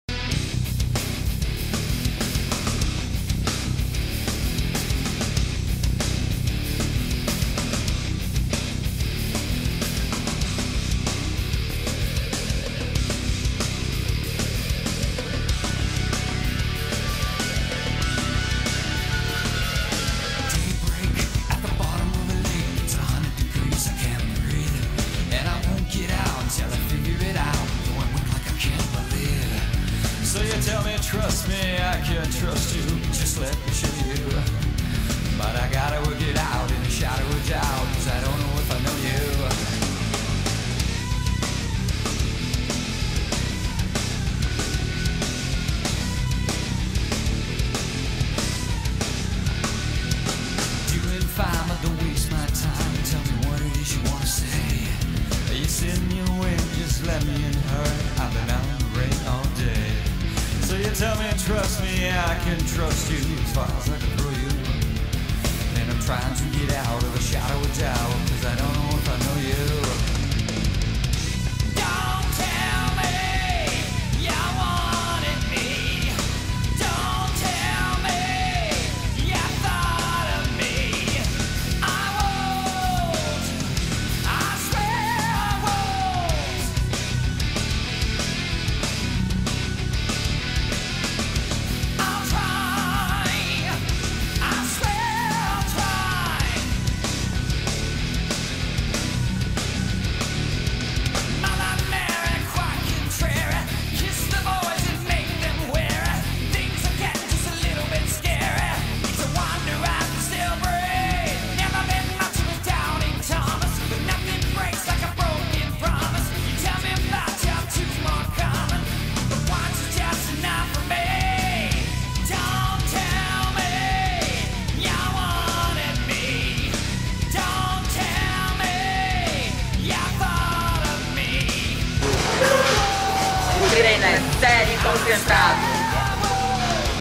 LEGS TRAINING COM FRAN ADRI E NICOLE ECCEL - Waldemar Guimarães
Curso - Construindo Programas de Treinamento com Competência (Periodização)